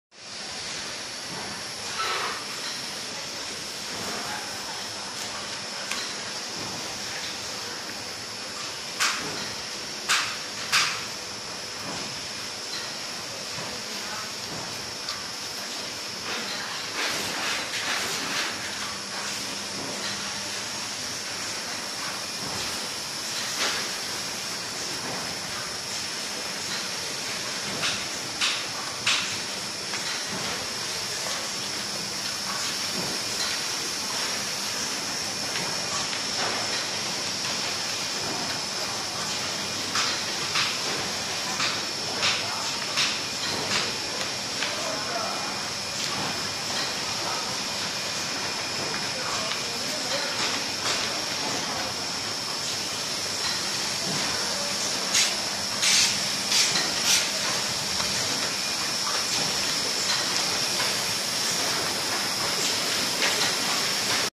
cardboard box covering paper production line
Luxury high quality cardboard packaging box covering paper production line, all box detail are custom made according to your requirements.